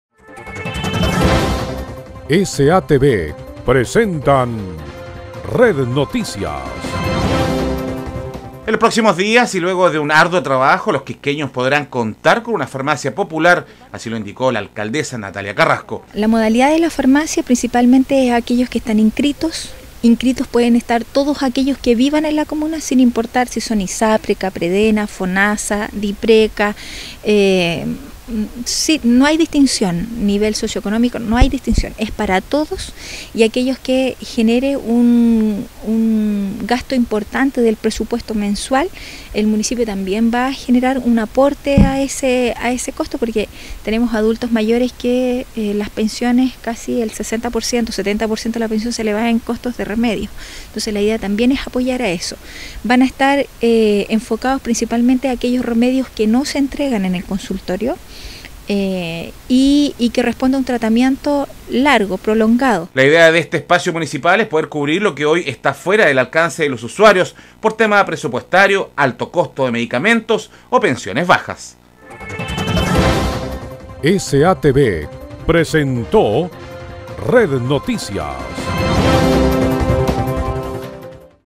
FARMACIA POPULAR EN EL QUISCO
En los próximos días y luego de un arduo trabajo los Quisqueños podrán contar con una farmacia popular así lo índico la alcaldesa Natalia Carrasco. La idea de este espacio municipal es poder cubrir lo que hoy está fuera de alcance de los usuarios por tema presupuesto alto costo de medicamentos o pensiones bajas